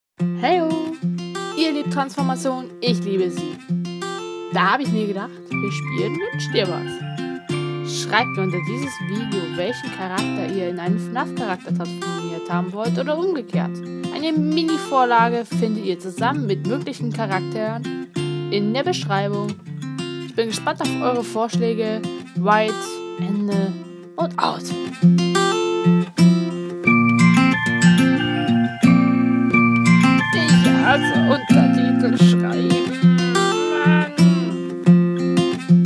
Wunsch Transformationen einreichen |Wünscht euch was
Heyo!
Ihr wollt eure 2 Lieblingscharakter transformiert haben? Hier seid ihr richtig!
(Okay der Satz war eventuell etwas übertrieben. Schließlich habt ihr ja nur eine relativ kleine Auswahl. Ja viel Spaß beim Stöbern)

Mini Vorlage:

(Begrüßung)
Ich hätte gerne (Person1) in (Person2) transformiert.
Dazu könnte ich mir (Beschreibung des Hintergrundes) vorstellen.

Der Hintergrund kann optional angeben werden.

Liste:
- Jeder Five Nights at Freddys Charakter
- Animal Crosing
- Pokémon
- Bendy and the ink machine
- Mario, Luigi, Yoshi
- Slenderman
- befreundete YouTuber (z.B A Swiss Gamer, "Wolki", Fnafi 94)
- andere Youtuber (Sev, Akeban)
- diverser meiner Oc´s

Auch möglich ist es möglich sie in einer vermenschlichte Form oder in einen komplett anderen Stil zu transformieren (z-B Animal Crossing, Mii´s ect)

Ist nix dabei? Nicht traurig sein! Vielleicht habe ich es nur vergessen aufzuzählen. Schreibt den ungelisteten Charakter einfach mit hin und den Ursprung dazu!
Zum Beispiel: Toy Freddy in Ruffy (One Piece)

Bitte beachtet, wenn ich die gewünschte Person nicht kennen sollte, dass es nicht klappt. Da ich die Charaktere wenigstens kennen möchte.

Nicht erlaubt:
- Gegenstände und Pflanzen
- Eigenwerbung durch eigenen OC

Ausgeschlossene Fandoms:
- Transformers (es ist zu kompliziert XD )

Habt einen schönen Tag!
White Engel